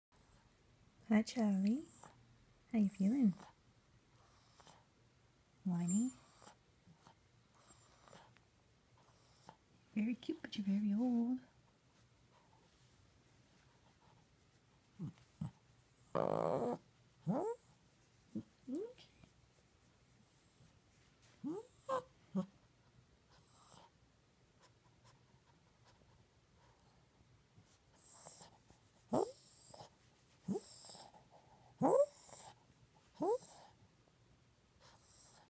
Charlie wants some cheese with his whine
So Charlie basically has doggie Parkinson's that is going to make it progressively harder for him to walk. It's clear he's not thrilled as the anesthesia from the X-rays is still wearing off 20+ hours later. This is the most quiet he's been since he spent last night howling constantly like a banshee. Poor little dude!